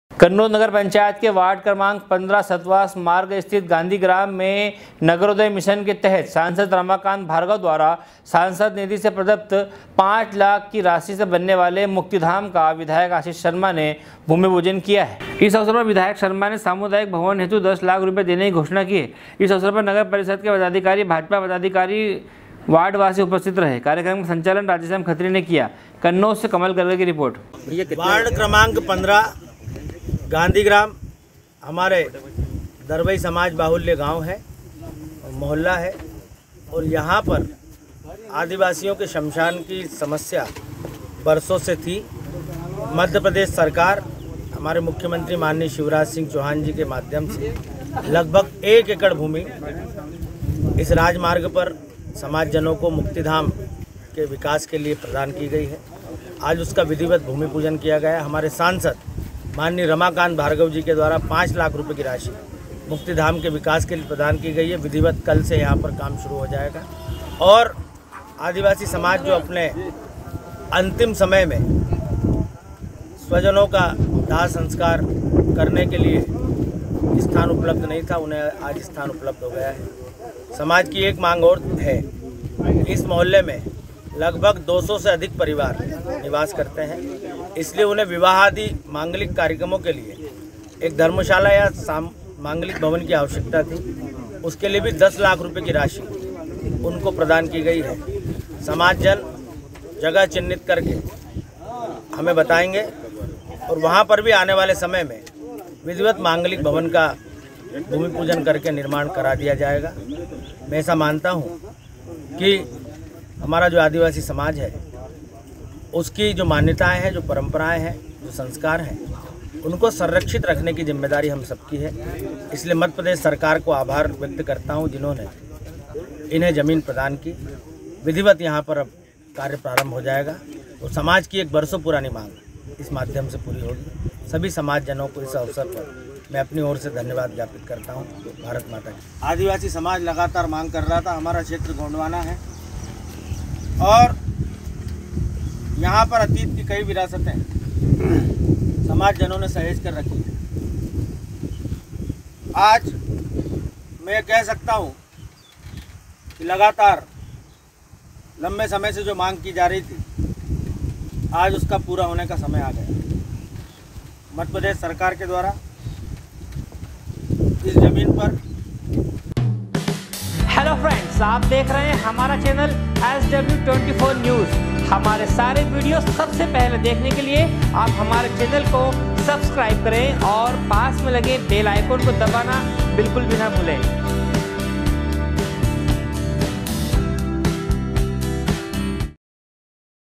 सांसद निधि से प्रदत्त 5 लाख की राशि से बनाने वाले मुक्तिधाम का विधायक आशीष शर्मा ने भूमि पूजन किया
SW 24 News Madhyapradesh
कन्नौद नगर पंचायत के वार्ड 15 सतवास मार्ग स्थित गांधीग्राम में नगरोदय मिशन के तहत सांसद रमाकांत भार्गव द्वारा सांसद निधि से प्रदत्त 5 लाख की राशि से बनाने वाले मुक्तिधाम का विधायक आशीष शर्मा ने भूमि पूजन किया।कन्नौद से कमल गर्ग की रिपोर्ट